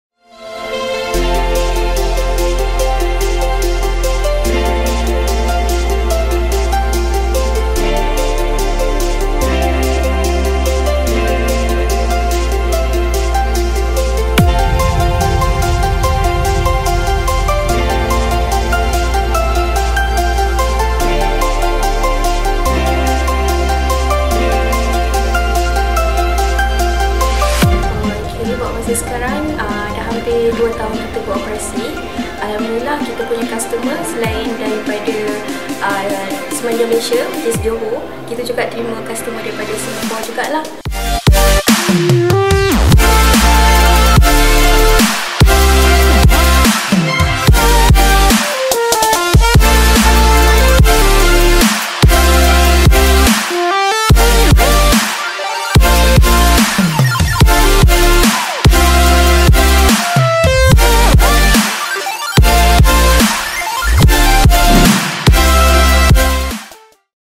UDA Business Center - A center for JB youth entrepreneur.
jbcara is an ongoing video making project aims to promote Johor Bahru as a liveable city.